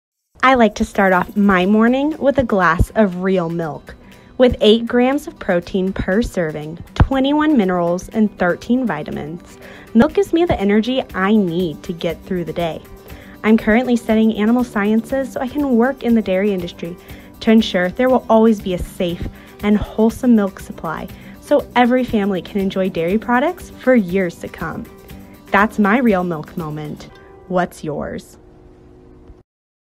Healthy Happy Cows Make Healthy Delicious Milk
Nutritious milk starts at the source, with healthy happy cows! Jumpstart your morning or get an afternoon boost with the nutrients your body needs.